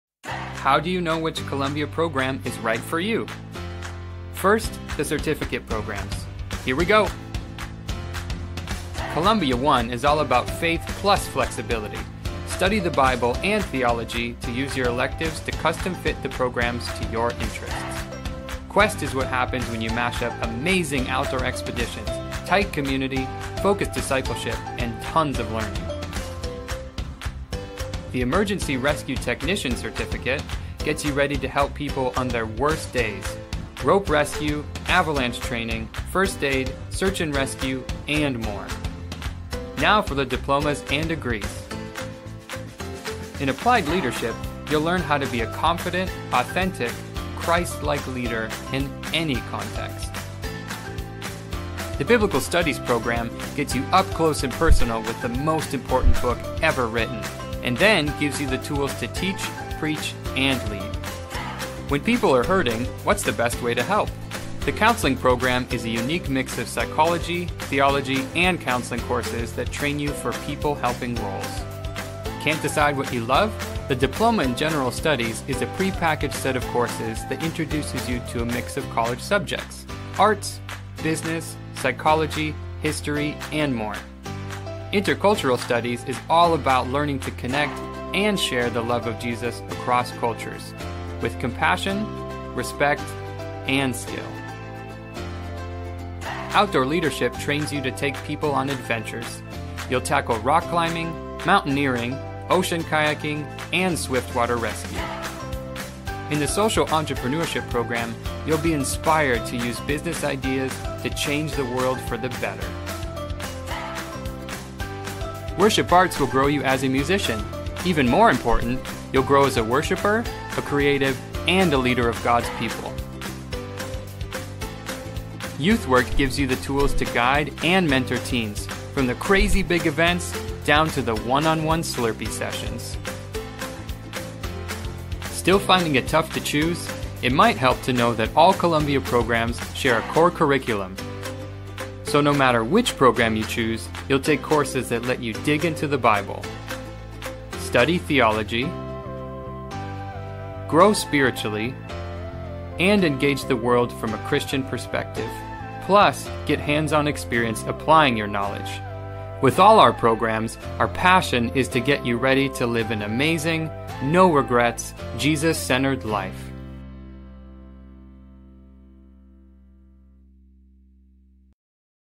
Which Columbia program is right for you?
A quick, fun overview of Columbia's one-, two-, and four-year programs, packed into a short video.